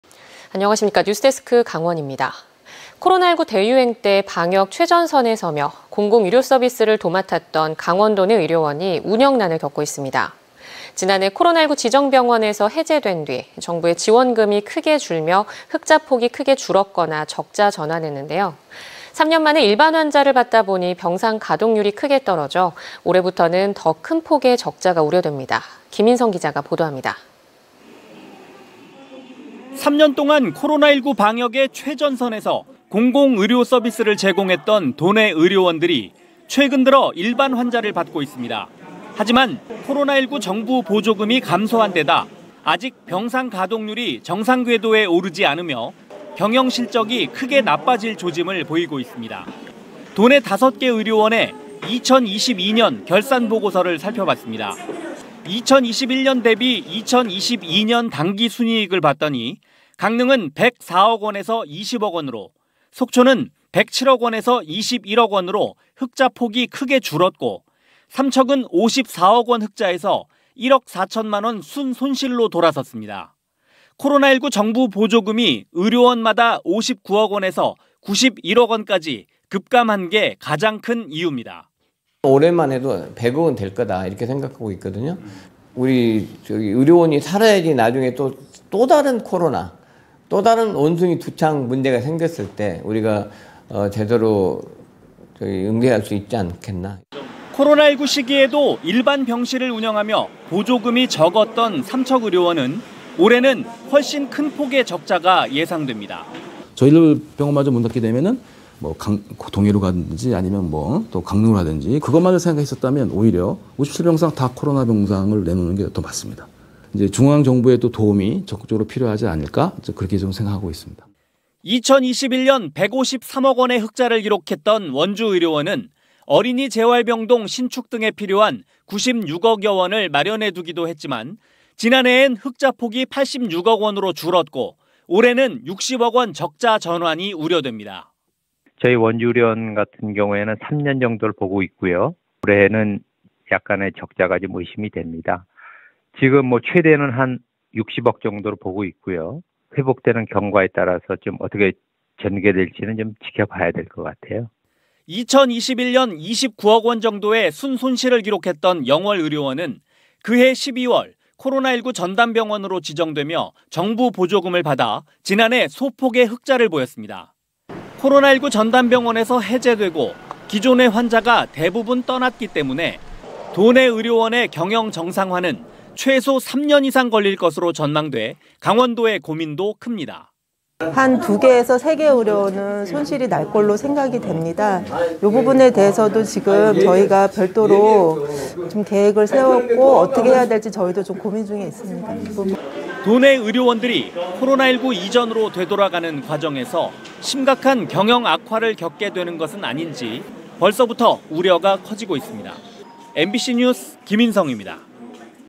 [뉴스리포트] 코로나 최전선 의료원, 적자 전환 우려 20230531
코로나19 대유행 때 방역의 최전선에 서며
공공 의료서비스를 도맡았던
강원도 내 의료원이 운영난을 겪고 있습니다.

지난해 코로나19 지정병원에서 해제된 뒤
정부의 지원금이 크게 줄며
흑자 폭이 크게 줄었거나 적자 전환했는데요,

3년 만에 일반 환자를 받다 보니
병상 가동률이 크게 떨어져
올해부터는 더 큰 폭의 적자가 우려됩니다.